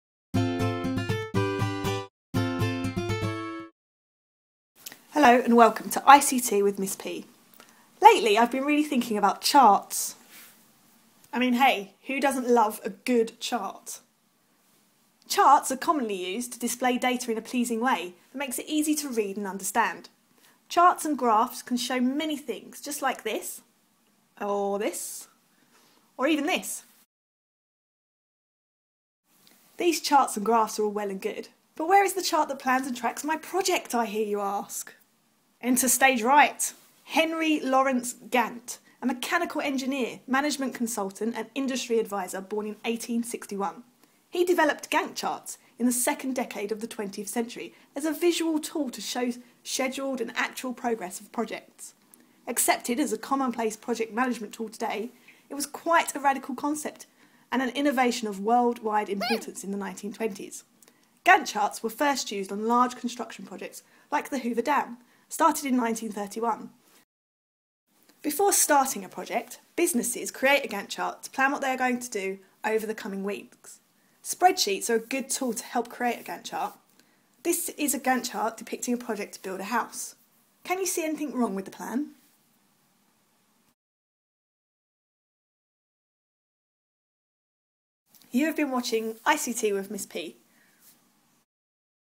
Gantt Charts
A short video explaining the concept of a Gantt Chart for KS3 Marketing Project in ICT.